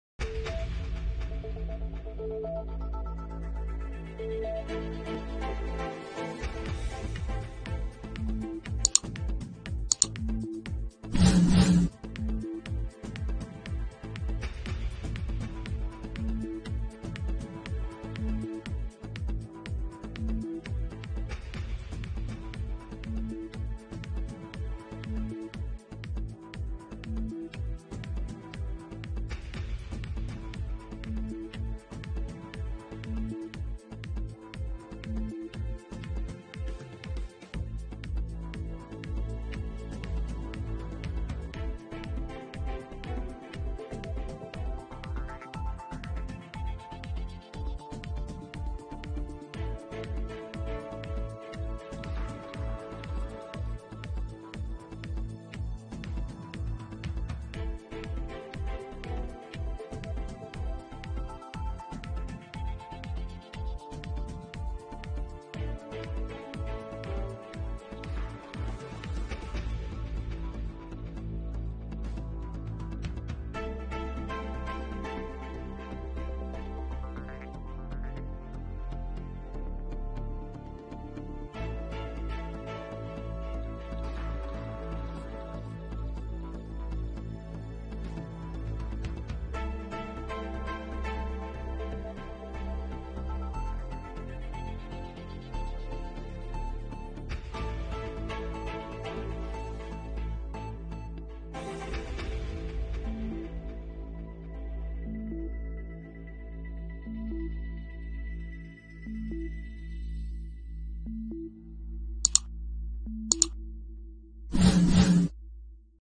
اسرا ئيل تفشل في اعتراض رابع صـاروخ حو ثي على أم الرشراش
اسرا ئيل فشل في اعتراض رابع صـاروخ حو ثي على أم الرشراش

مسلسل ماء الذهب
اشتباكات_ البحر_الأحمر
مواجهات_ البحر_الأحمر